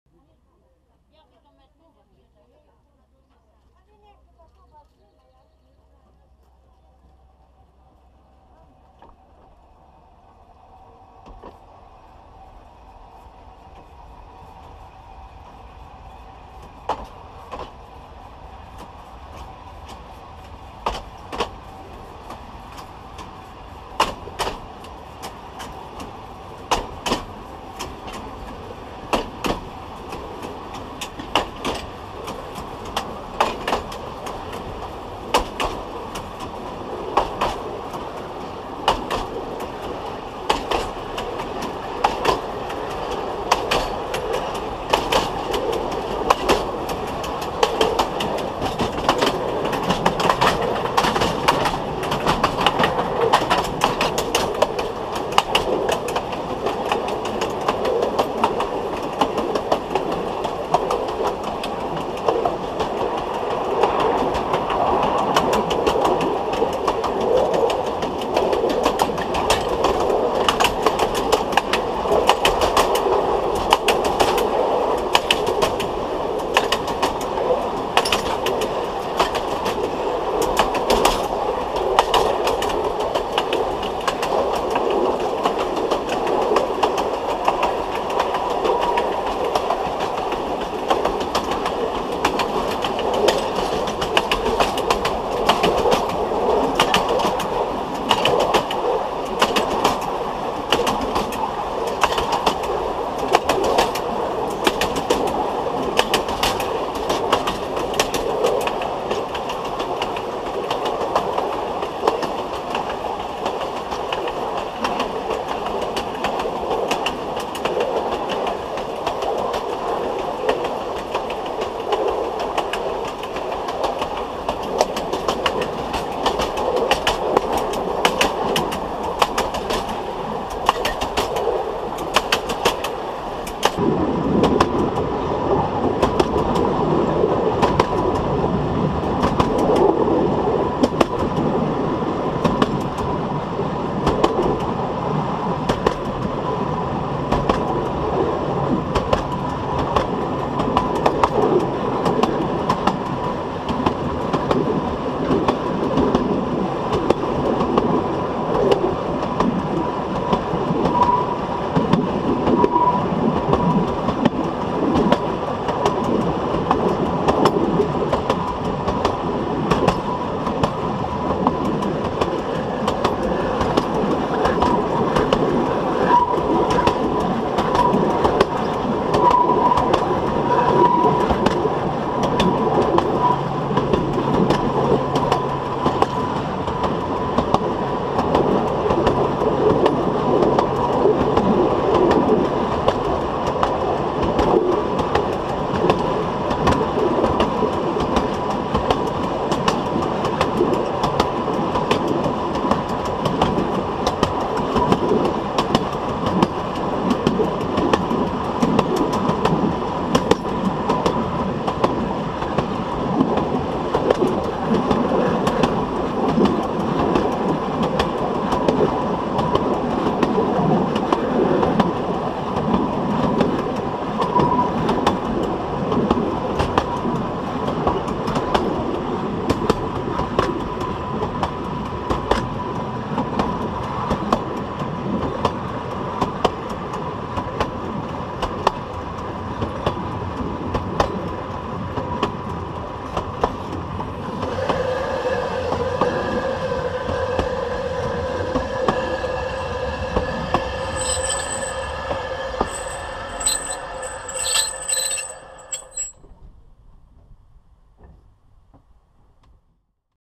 Undercarriage view of a narrow gauge train.
A view under the running coach on the narrow gauge railway Septemvri - Dobrinishte with clicking sound of the rail joints.